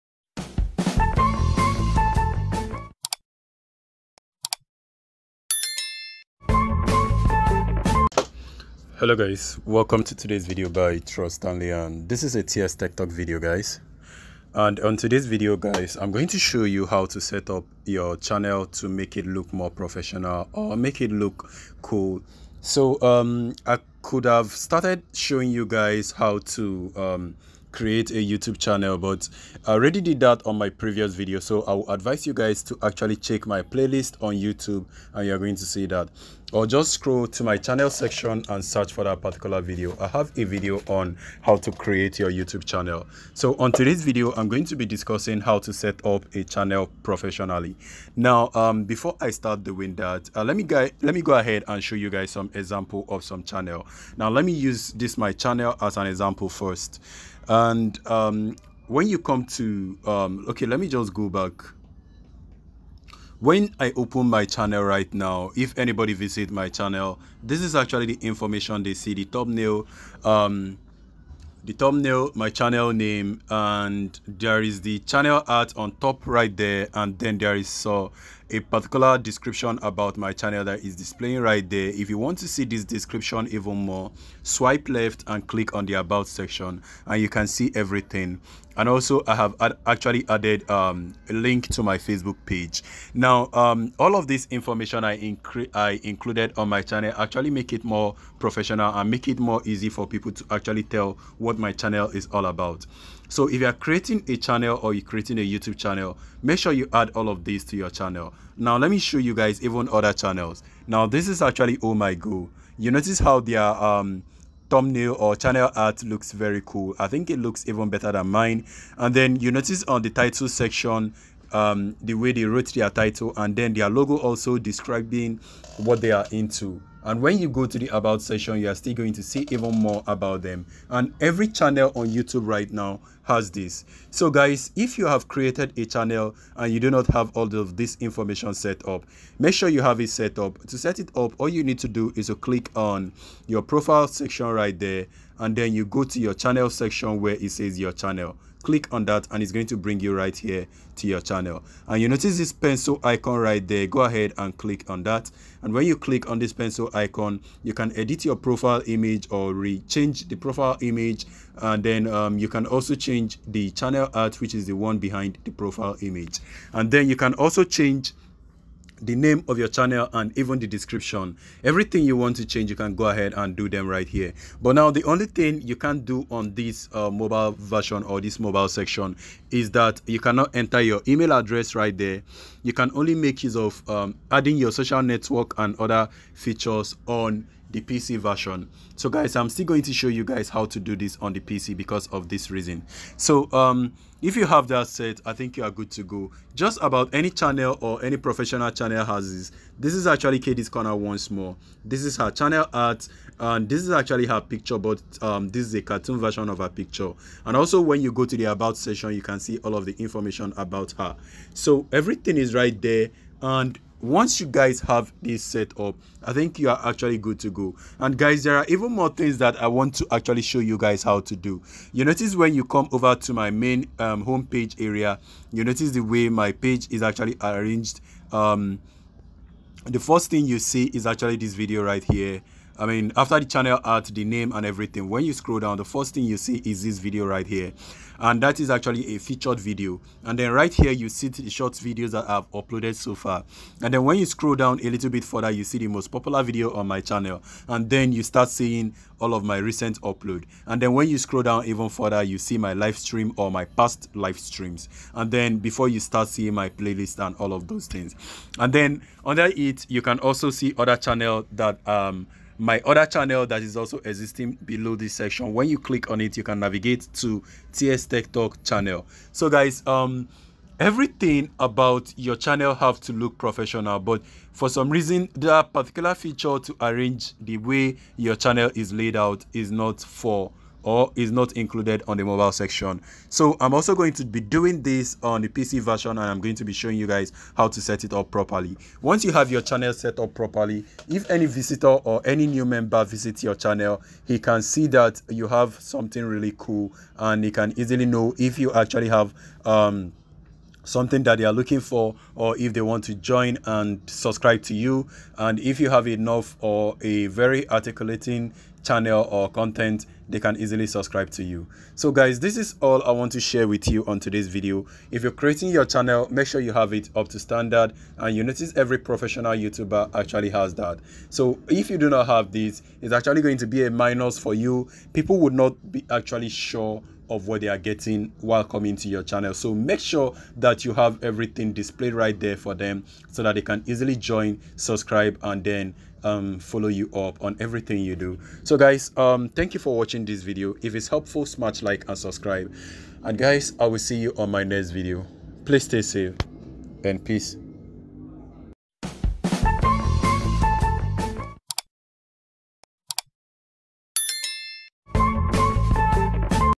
How a Youtube Channel Should Be Set up With A Mobile
i will discuss How a Youtube Channel Should Be Set up With A Mobile, when you are creating a youtube channel ths is what you should do to make it look professional.